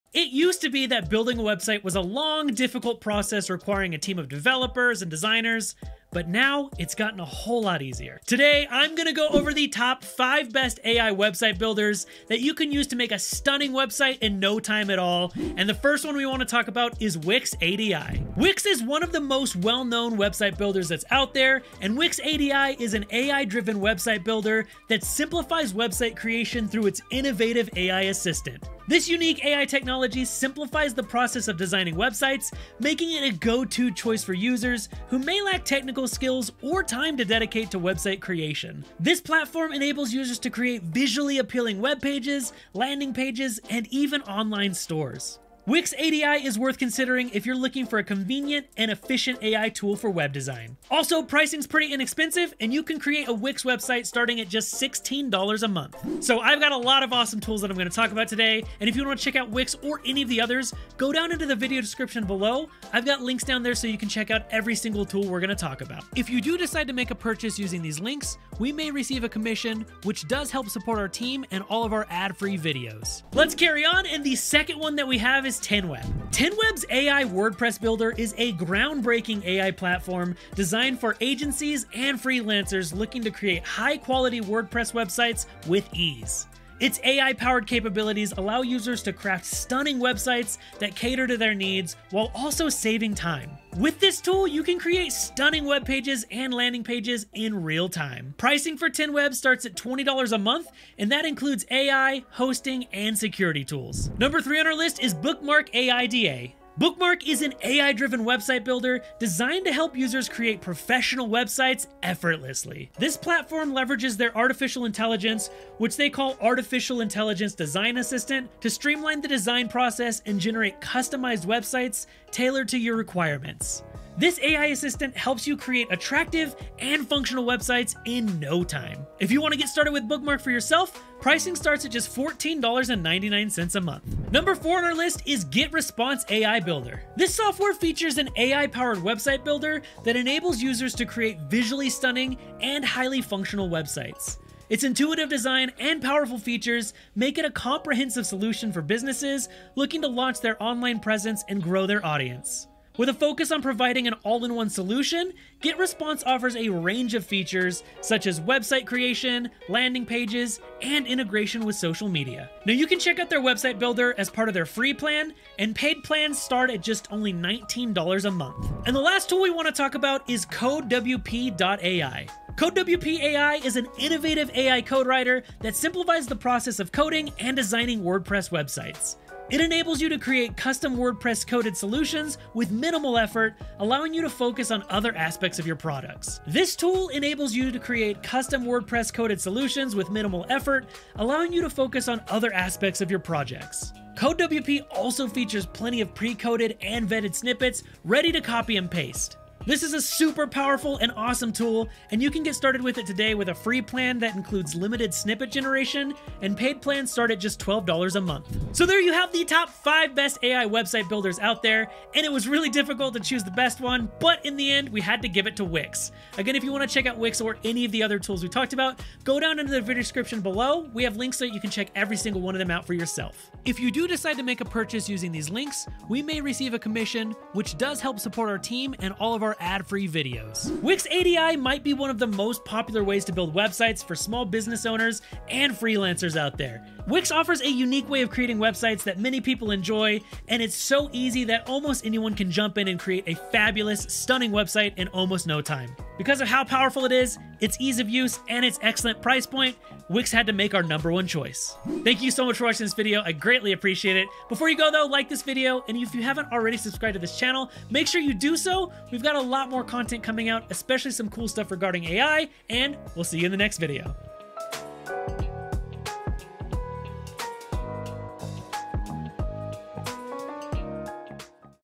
5 Best AI Website Builders in 2024
Have you spent hours and hours designing websites on platforms that are hard to use? Are you looking for a faster, more efficient way to create a visually stunning online presence? It’s time to embrace the future with the best AI website builders that can give you a helping hand. In this video we go over the 5 Best AI Website Builders in 2024!

Timestamps ⏰
0:00 Intro
0:16 Wix ADI
01:13 10Web
01:43 Bookmark AiDA
02:14 GetResponse AI Builder
02:48 CodeWP.ai
03:28 Our Favorite